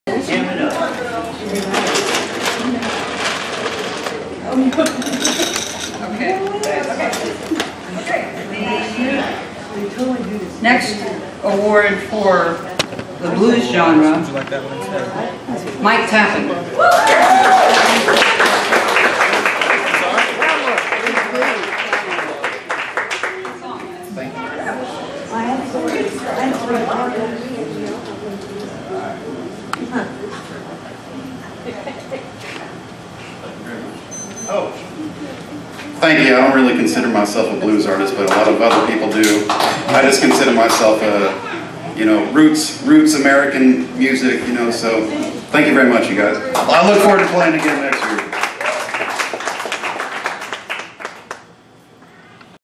Winner, Blues, Mike Tappan @ SuwRK Songwriting Contest 2018-06-23
WWALS Watershed First Annual Suwannee Riverkeeper Songwriting Contest Finals

Videos by John S. Quarterman for WWALS Watershed Coalition (WWALS) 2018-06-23.